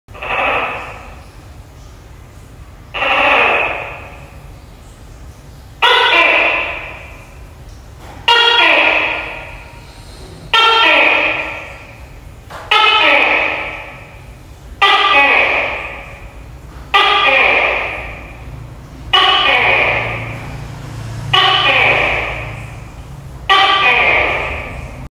Âm Thanh Tắc Kè Hay Nhất Rõ Nhất, Tiếng Kêu Tắc Kè Trong Đêm Tối by Phạm Tuấn Kiệt TV
No coppyright
Bản quyền âm thanh hình ảnh thuộc Phạm Tuấn Kiệt TV